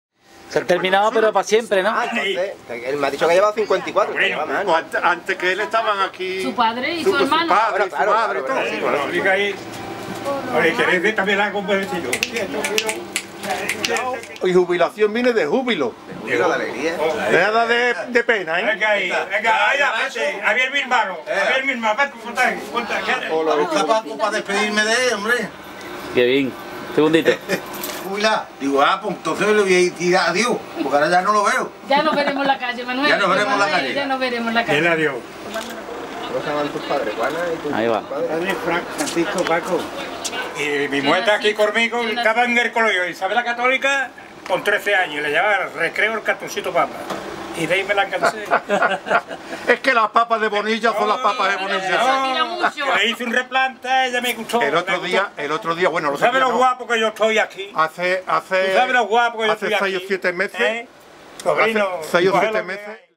Adiós a los churros con más duende
Paco Bonilla, el famoso y veterano churrero de La Plata, deja por jubilación su puesto tras 54 años.